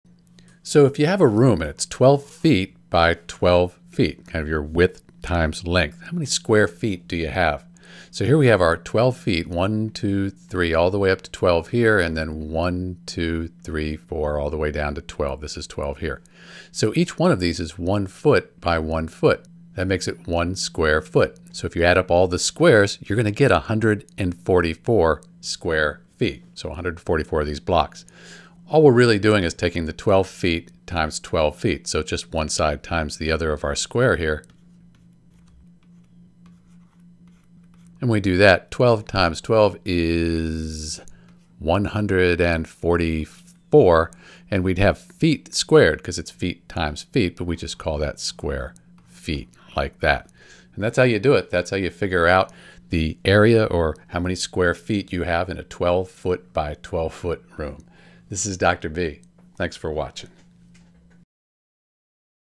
How many square feet are in a 12ft by 12ft area?
In this video, we'll learn how to calculate the area of a 12x12 ft square (or any rectangular area). To find out how many square feet are in a 12x12 ft area, you simply multiply the length by the width. In this case, both the length and the width are 12 feet.

To make it clearer, imagine a grid that is 12 feet on each side, like graph paper. Each small square on this grid represents one square foot. If you look at the entire grid, you'll see there are 144 of these one-square-foot areas.

So, if you have a 12x12 ft room or building, the total area is 144 square feet, which is what we get when we multiply 12 ft by 12 ft. This method works for any rectangle or square—just multiply the length by the width to find the total area in square feet.